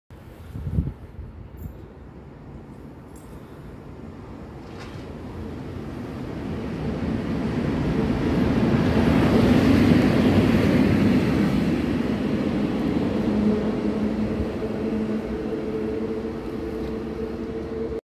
EP07-1507 PKP Cargo z transportem CZ LOKO ||Kłodzko Miasto
Witajcie . pojechałem do Kłodzka by nagrać skodę ČD Cargo która jechała do Międzylesia  luzem . Gdy wszedłem na stację zobaczyłem światło zielone , a z drugiej strony światła a więc szybko ustawiałem  telefon na statywie i nagrałem ten oto duet EU07-1507 i CZ LOKO .
stacja: Kłodzko Miasto
Pozdrawiam